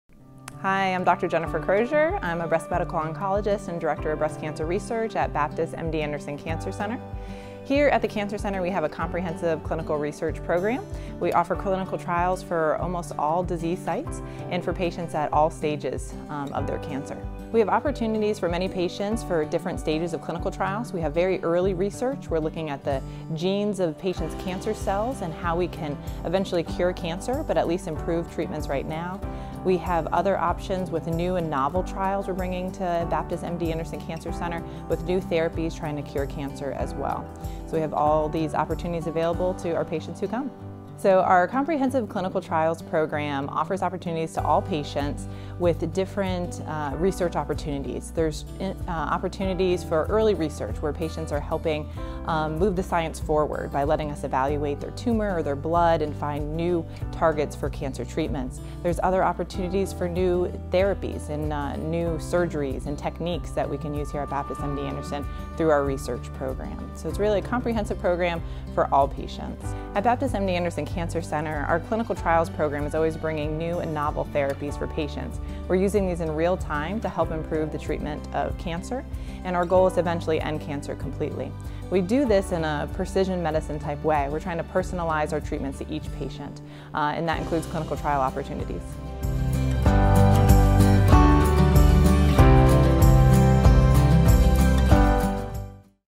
Survivorship Week Expo: Clinical Trials
Jennifer A. Crozier, a medical oncologist with Baptist MD Anderson Cancer Center, discusses the comprehensive clinical trials research program that offers research opportunities for all patients at all stages of their cancer, with the goal to endcancerJAX completely.